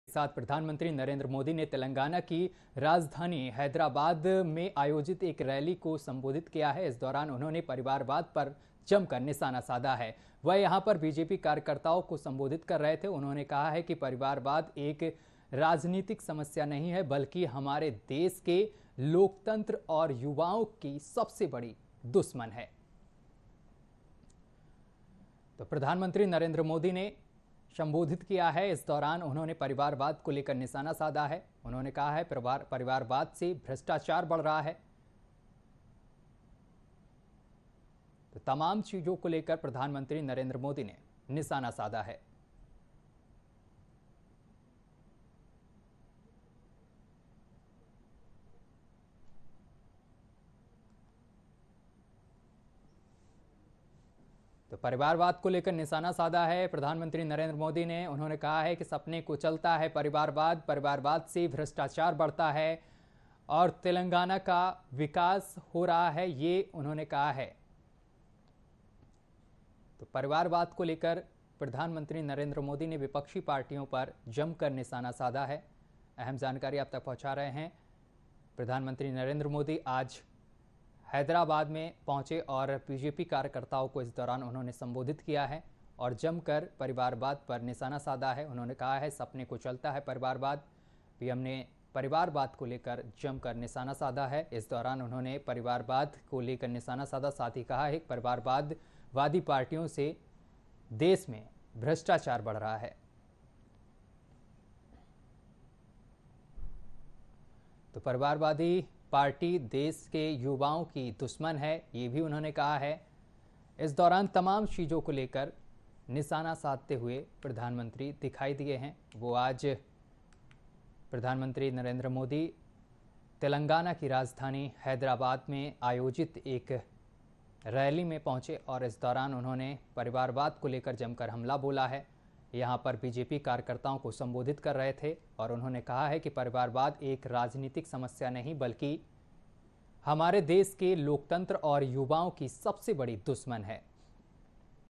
PM Modi Hyderabad: PM मोदी ने साधा परिवारवाद पर निशाना, जानें क्या बोले
PM Modi Hyderabad: मोदी हैदराबाद पहुंचे. जहां उन्होंने आईएसबी हैदराबाद के 20 साल पूरे होने के उपलक्ष्य में आयोजित समारोह में भाग लिया। इस दौरान पीएम मोदी ने बीजेपी कार्यकर्ताओं को संबोधित करते हुए कांग्रेस समेत तमाम विपक्षी दलों को निशाने पर लिया. उन्होंने कहा कि, देश में परिवारवादी पार्टियां राज करके जनता को लूटना चाहती हैं. लेकिन अब तेलंगाना के लोग बदलाव चाहते हैं।